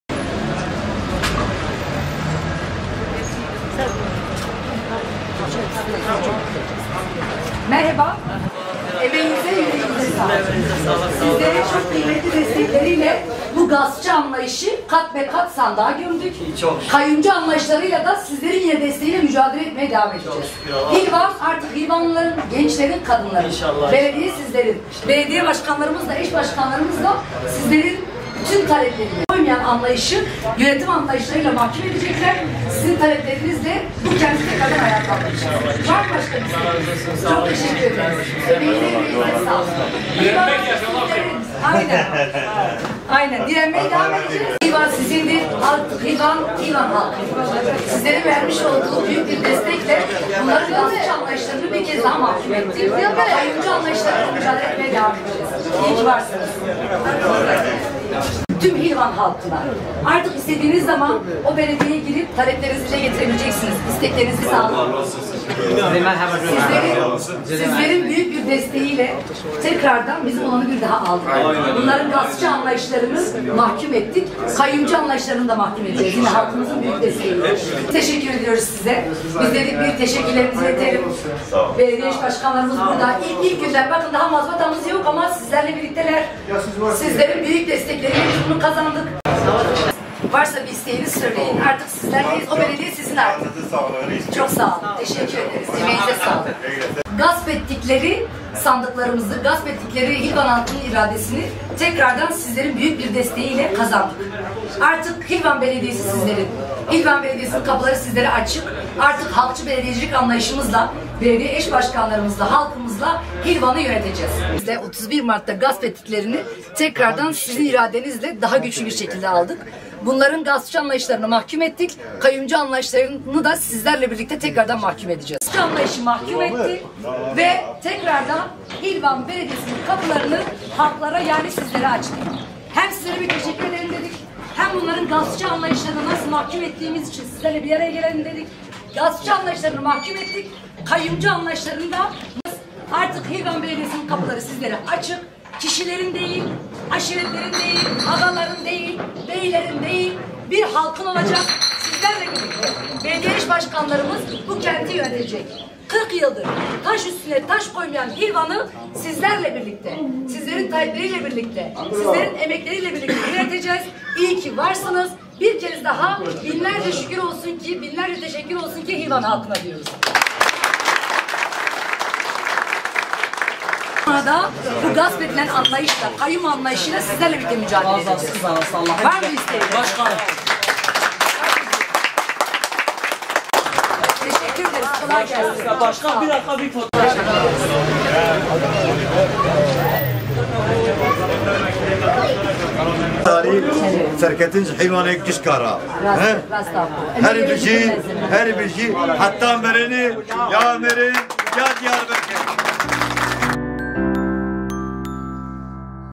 İKİNCİ KEZ KAZANAN EŞBAŞKANLARDAN TEŞEKKÜR ZİYARETİ
Yenilenen seçimleri kazanan Curnê Reş Belediyesi Eşbaşkanları Serhan Paydaş ile Garip Yeşil, esnafa teşekkür ziyaretinde bulundu.